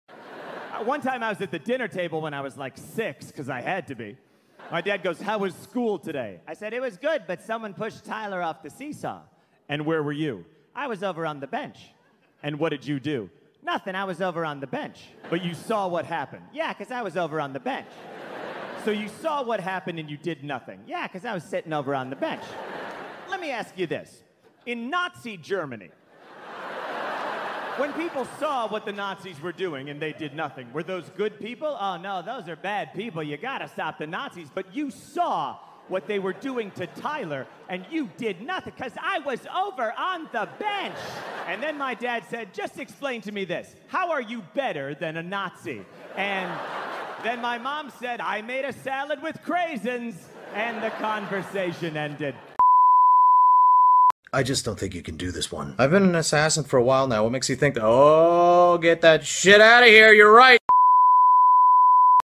Any was over on the bench | [Spy x Family Animatic]
It's been a while since I made a Spy x Family animatic, I would've posted this 2 weeks sooner but I was busy with collage.

Credits
Characters are owned by: Tatsuya Endo

Audio by: John Mulaney